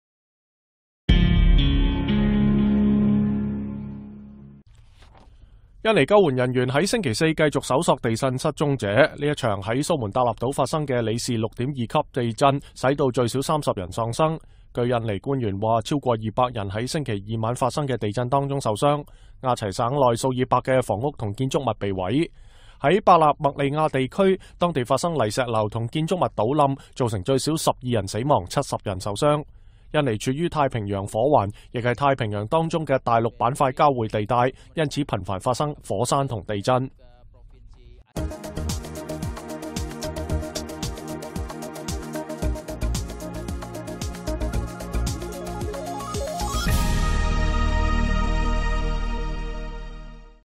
2013-07-04 美國之音視頻新聞: 印尼救援人員繼續搜索蘇門答臘地震失蹤者
印尼救援人員在星期四繼續搜索地震失蹤者。這場在蘇門答臘島(Sumatra)發生的里氏6.2級地震使到最少30人喪生。